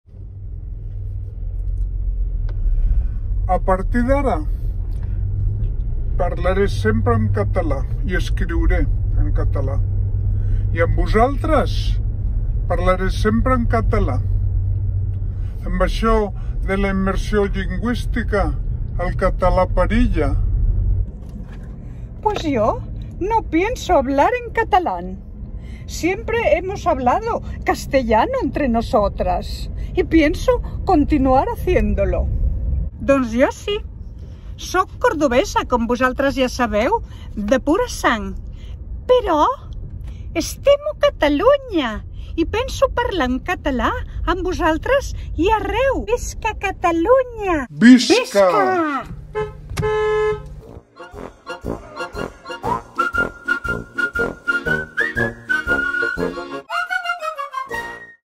Immersió lingüística
Esas chicas de 70 hablan sobre la inmersión lingüística